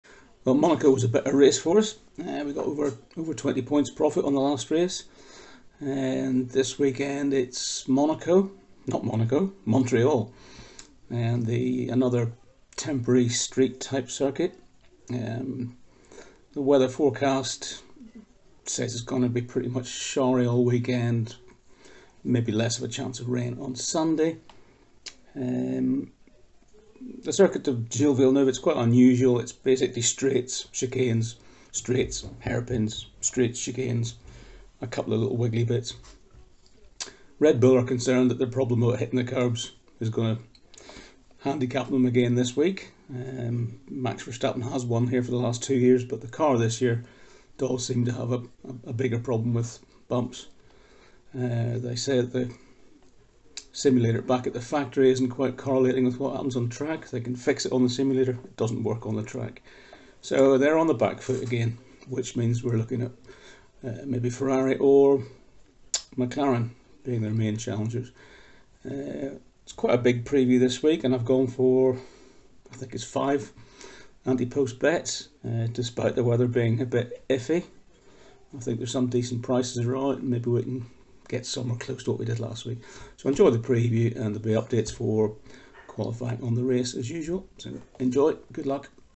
2024 Canadian GP Betting Preview | More GLORY for Leclerc? | James Punt
We head to Montreal for the CanadianGP this weekend and our F1 expert @JamesPunt has an outright betting preview.

He likes the look of 5 ante-post bets, get his FREE tips and in depth, team by team analysis on our blog.

Qualifying & Raceday Updates will be posted here in the comments on Saturday and Sunday.